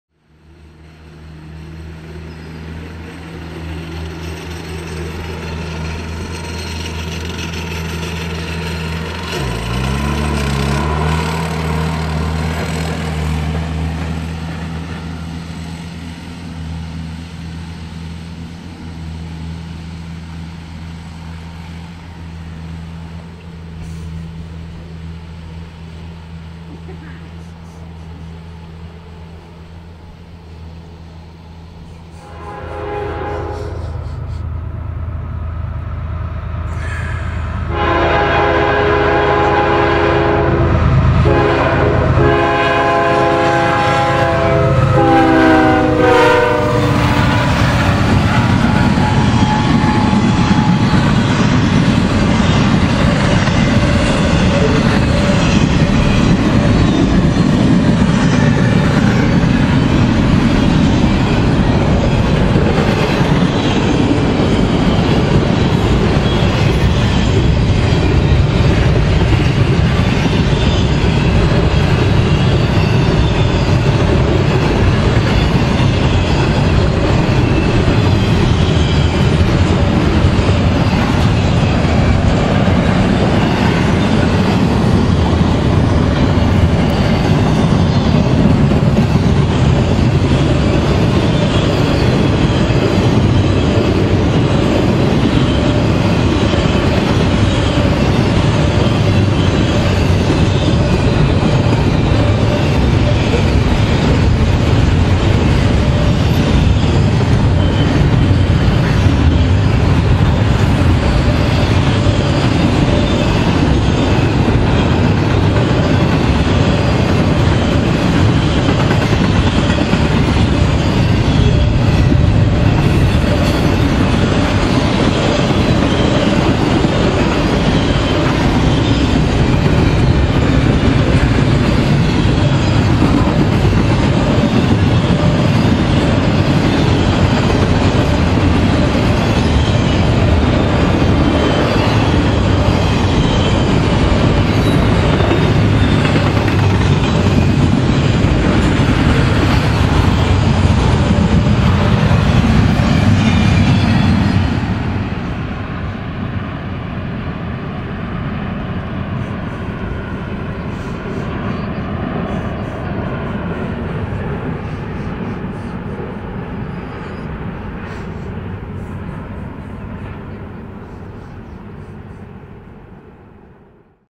(17) CSX Q165 @ Westfield June 21 2017
A west bound CSX double stack container train, Q165 (CP 143 while in Canada) passes Westfield, New York. June 21 2017.